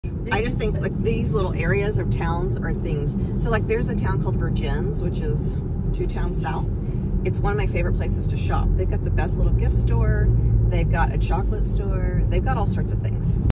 Hidden Vermont GEM! Vergennes, shopping, nature, and more!
Nestled in the heart of Vermont, the picturesque town of Vergennes offers an idyllic shopping experience that is not to be missed. With its charming streets lined with historic buildings and independent shops, this small town exudes character and warmth✨

if you were looking for a great place for some spring shopping, Vergennes is one of my secret favorites 🌷